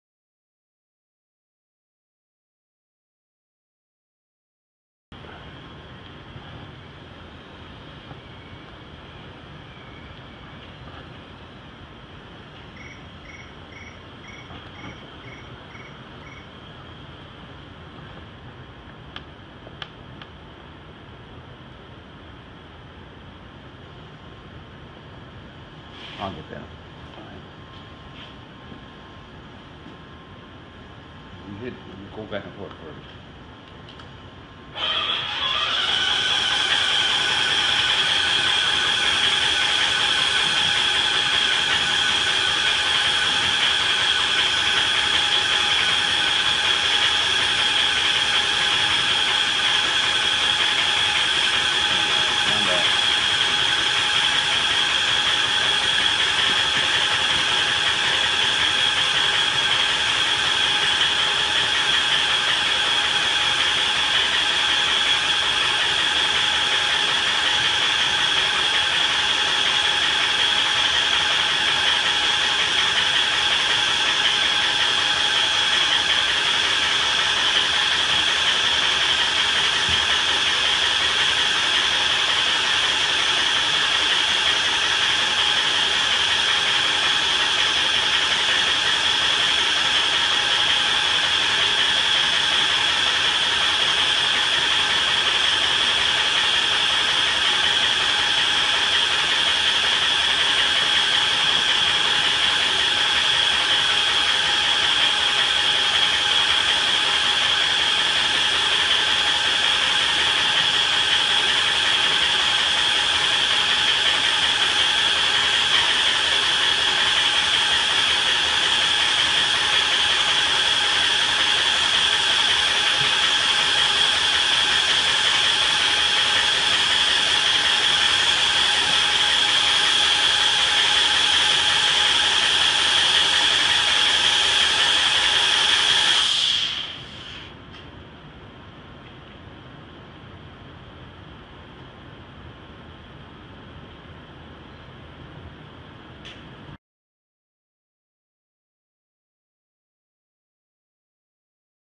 Encapsulation of Fiberglass Duct Board Using Duct Armor
Using Duct Armor to encapsulate fiberglass duct board in an above ground air duct